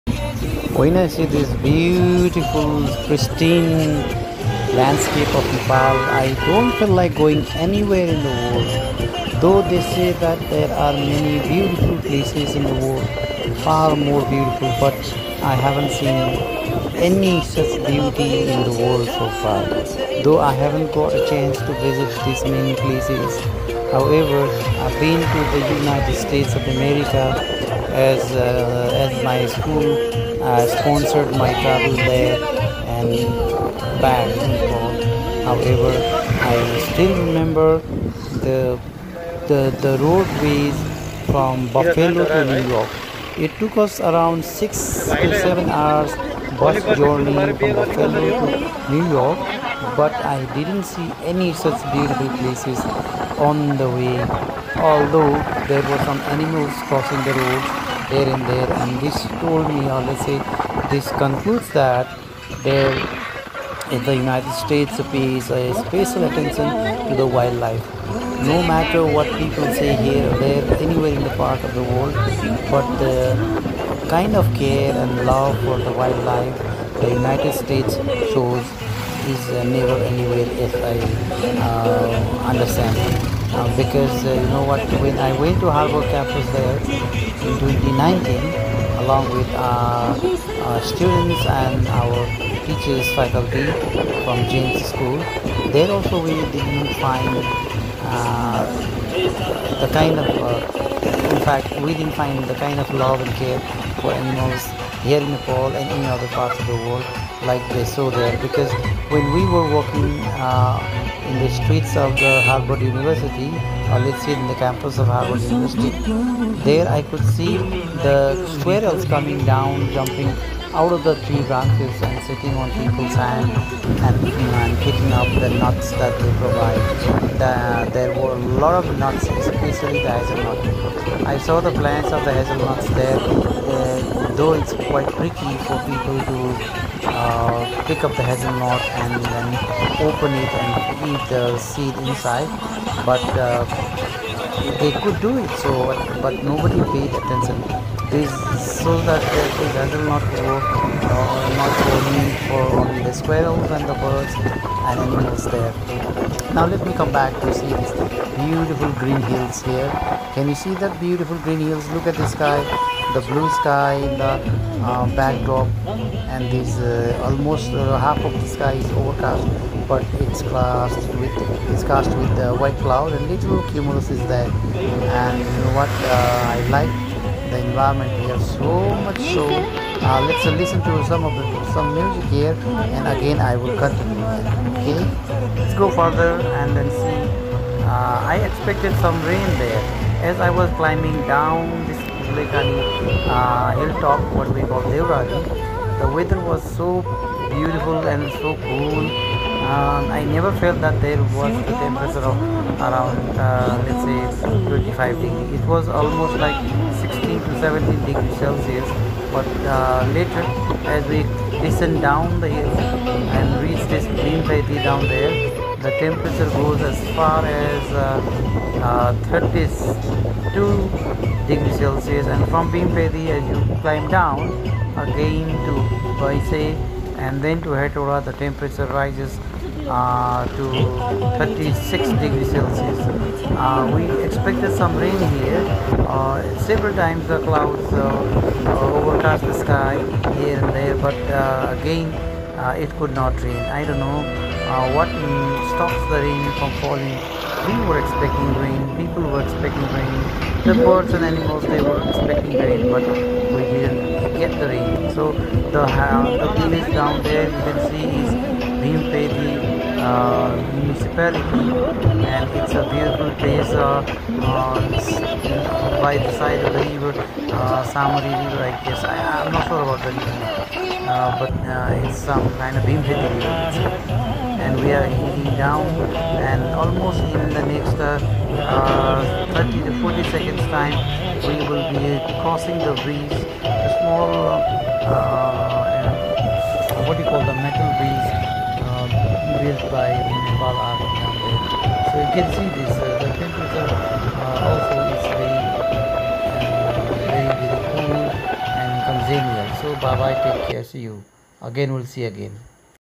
Thru Nature's Lap
Another trip to Hetauda through Kulekhani Nepal. The weather this time has meticulously cleaned the skyscape and the landscape below enthralls you like anything.
Please travel through the thunderstorms of the thick and thin snaky road that twirls and twists every 30 to 50 meters. You will certainly have a heavenly experience. Watch and write if I can be untrue. I hereby invite you to take a test ride through this road too